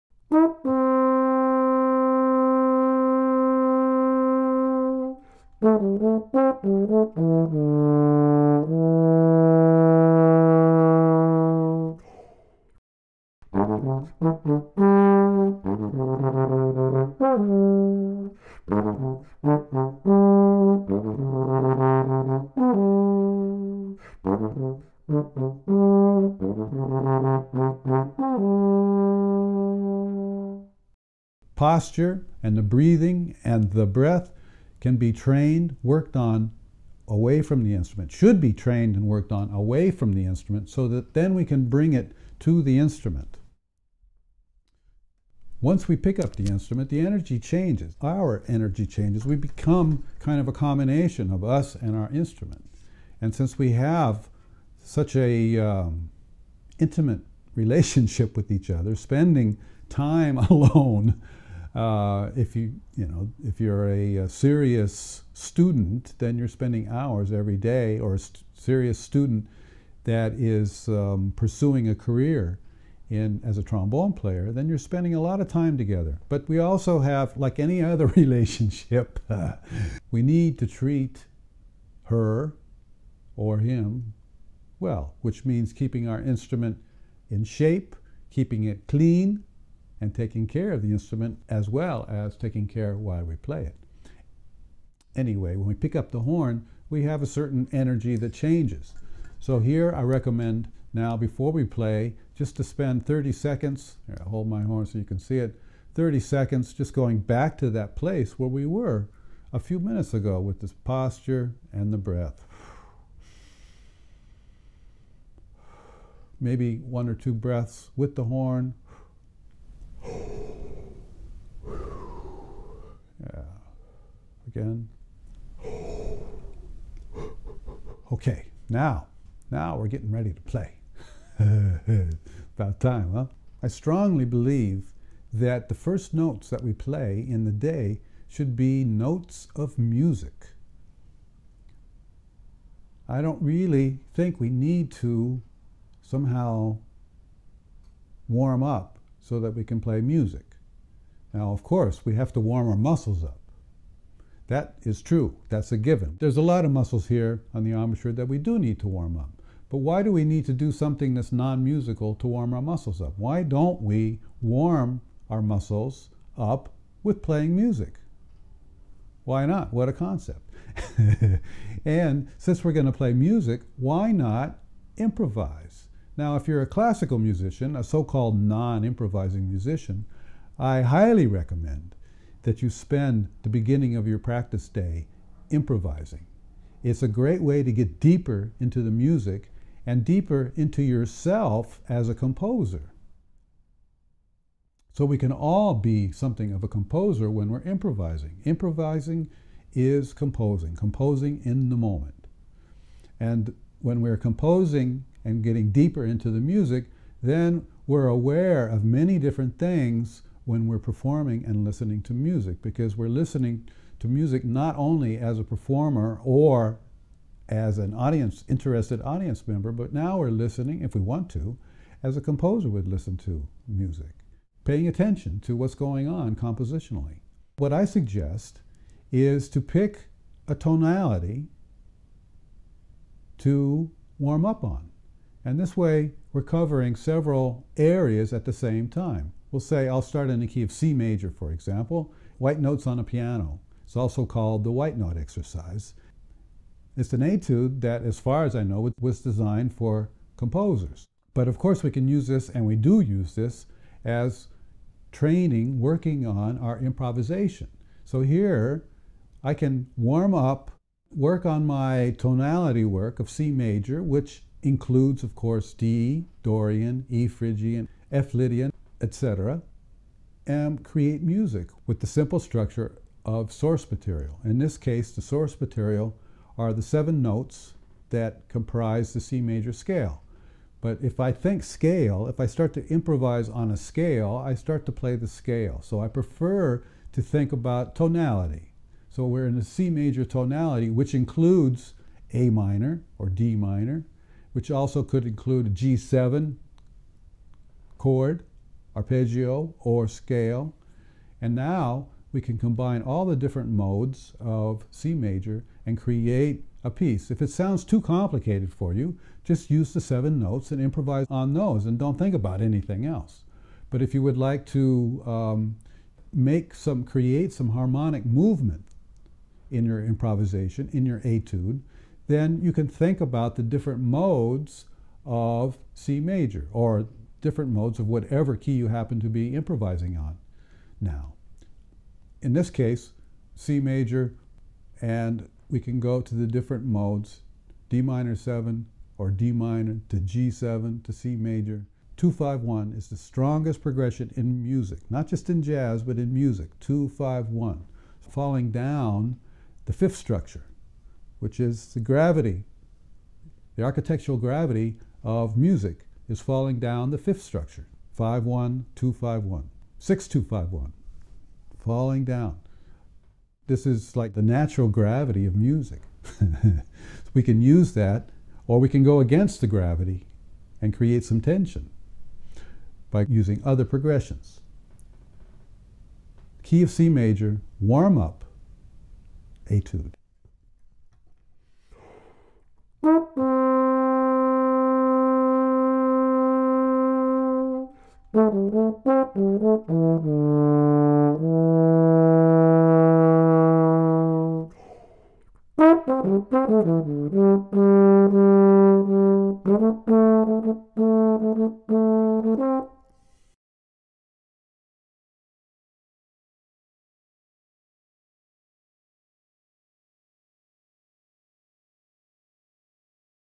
Creative Practicing - Practice Creatively, Part II of V
Part II Intro to C Major Etude

Ed Neumeister presents a video demonstration lecture series covering the subjects of: Posture, breathing, warming up the muscles, developing technique, improvisation and composition

Creative Practicing - Practice Creatively is a five part video series by master Composer/Trombonist Ed Neumeister.  Ed discusses and demonstrates ways to make "doing" more focused, enjoyable, efficient and effective.  He uses practicing an instrument as an example of how to remain focused on the task of the moment.  Using breathing, improvisation and composition to develop technique on the instrument.